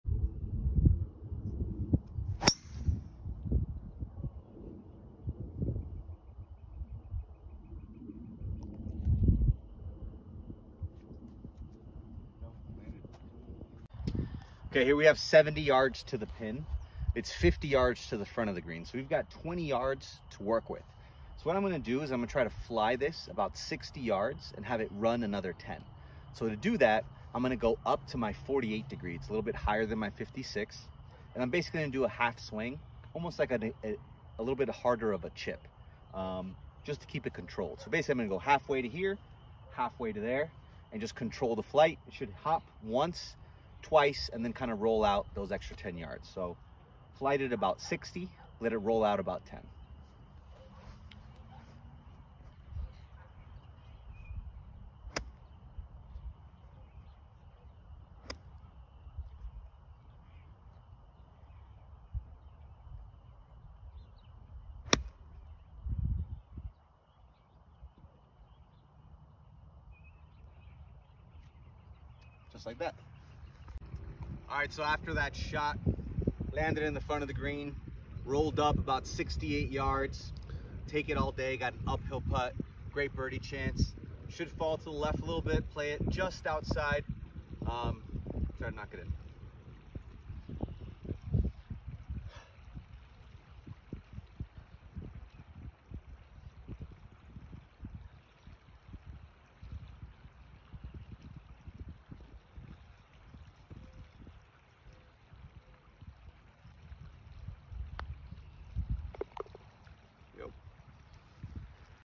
How to dial in your wedges for all those 100 yards shots and in!
Those short shots always confuse amateur golfers. The goal is to use ALL of the wedges in your bag. It is ok to have a full swing, 3/4 swing, half swing, or just a swing that feels like a long chip…to use for those shots. Here I use my 48 degree which I hit about 130 full. At 70 yards, I know my half shot with a 48 is going to fly 60 and then bounce and roll another 6-10 yards, with a simple half swing.